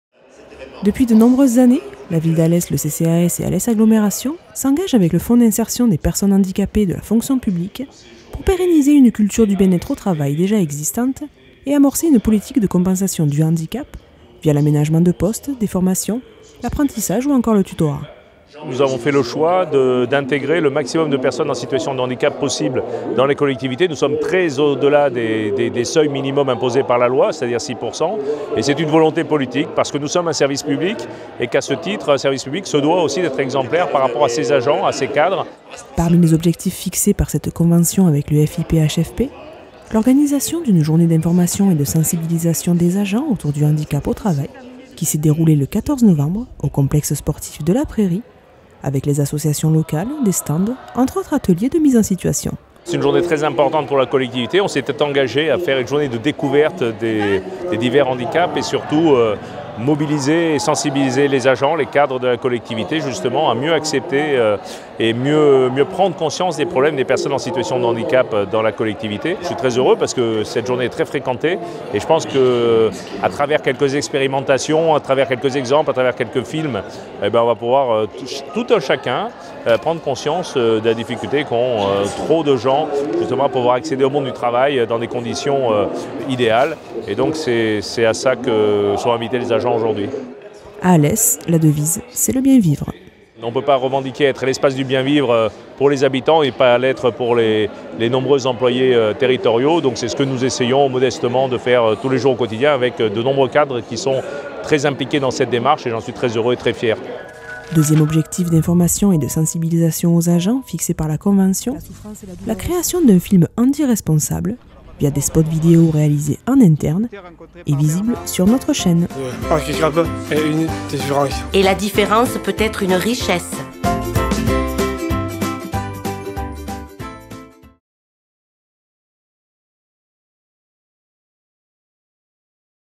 La Ville d'Alès, Alès Agglomération et le CCAS engagés en faveur du handicap au travail
Depuis de nombreuses années, la Ville d'Alès, le CCAS et Alès Agglomération s’engagent avec le Fonds d’Insertion des Personnes Handicapées de la Fonction Publique pour pérenniser une culture du bien-être au travail déjà existante et amorcer une politique de compensation du handicap. Dans ce contexte, une journée d'information et de sensibilisation au handicap au travail était organisée à destination des 2 500 agents, le 14 novembre 2017.